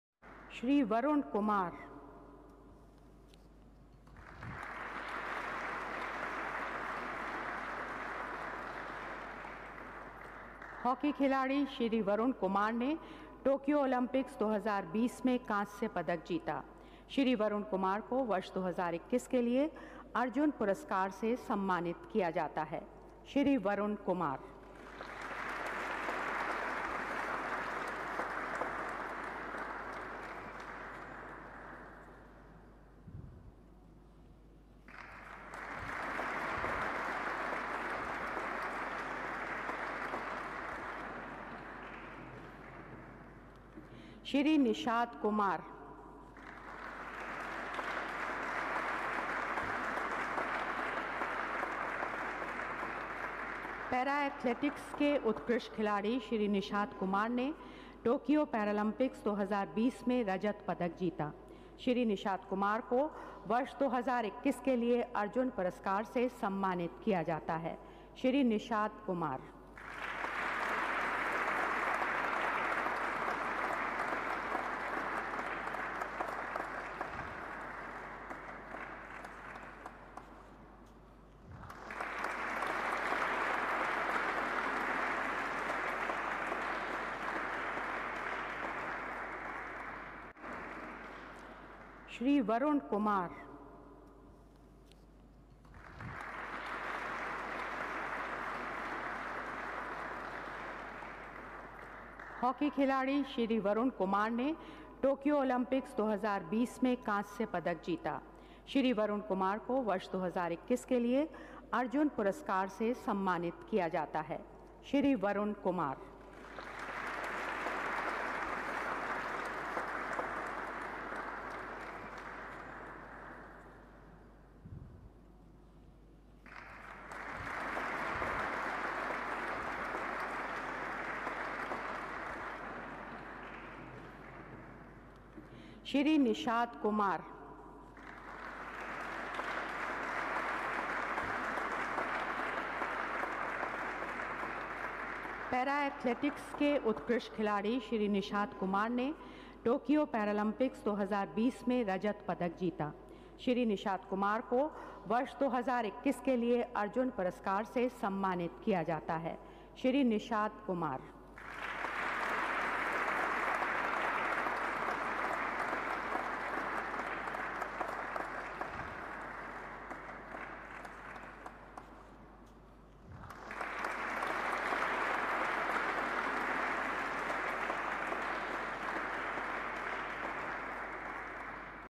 हिमाचल के दो बेटों ने पाया ‘‘अर्जुन अवार्ड’’, बढ़ाया प्रदेश का मान
हिमाचल  के दो बेटों ने पाया ‘‘अर्जुन अवार्ड’’, बढ़ाया हिमाचल का मान